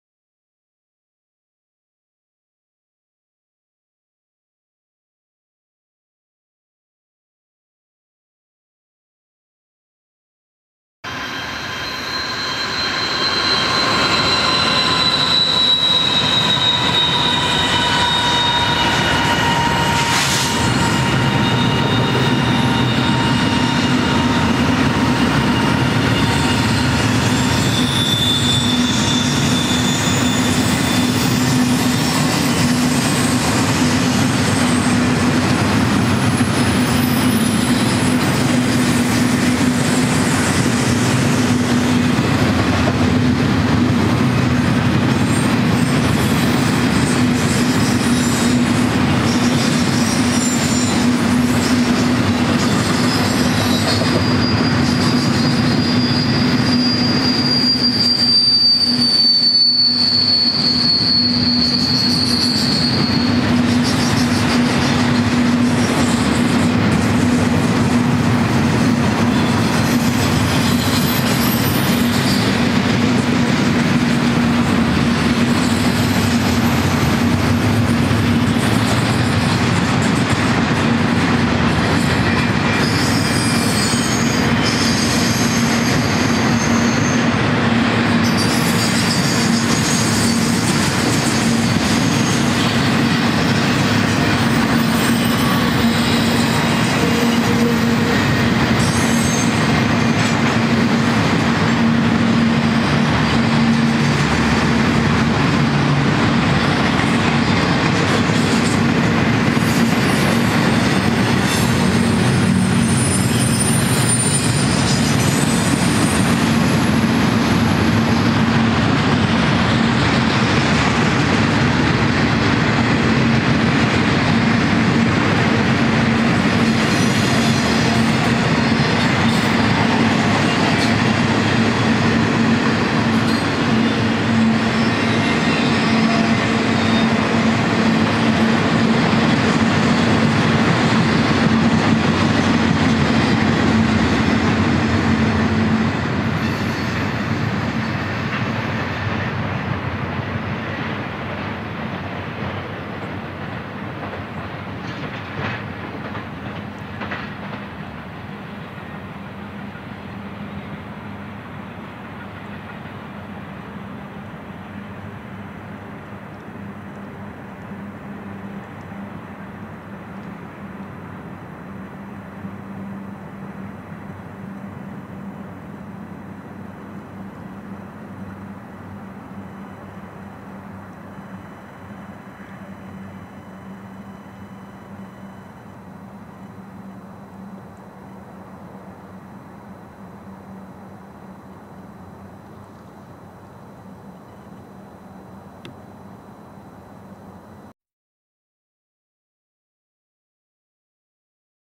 3820 Train BNSFRon D. High-Desert Railfanning
BNSF 3820 ET44C4

Double Stack & Trailers & Single Containers.
Shippers Include:  J.B.Hunt Intermodal, Swift, Prime (Amazon), YRC Freight, Yellow Freight, Walmart.com, Fed-Ex,   Rail Freight Transport in action.

BNSF 3820 + 7506 + 6562 + 5015  All GE Locomotives.